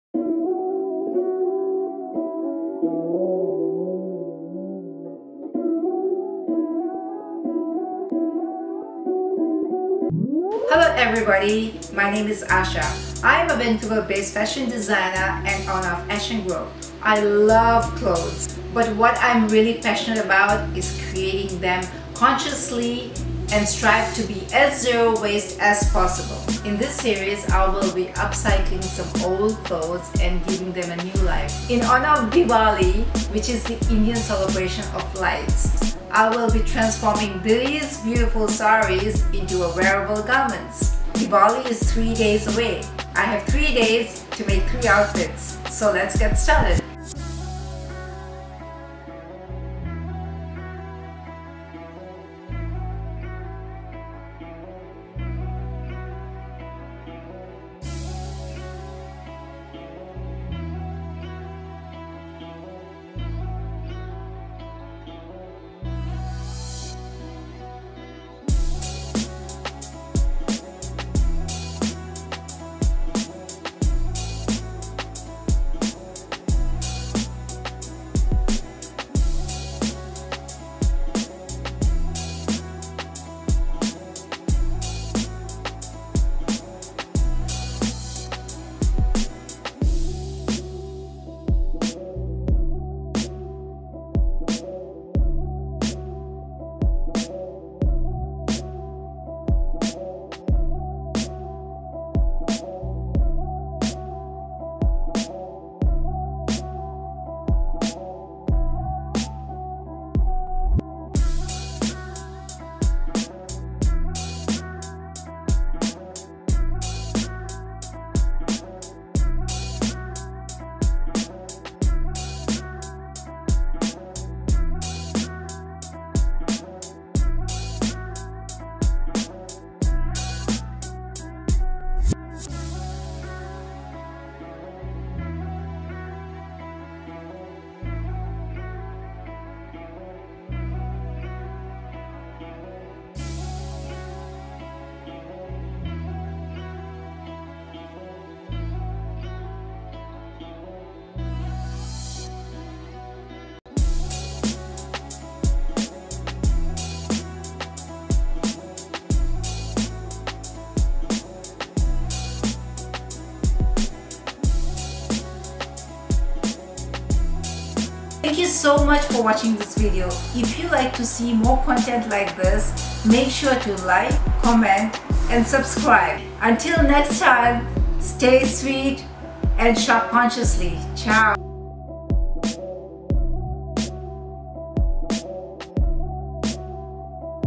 UPCYCLE SERIES// #1 Diwali
This series is dedicated to upcycling old clothes you may have lying around. I challenged myself to make three different Indian-style outfits in the three days leading up to Diwali. Let me know what you would have done in the comments!

Credits!!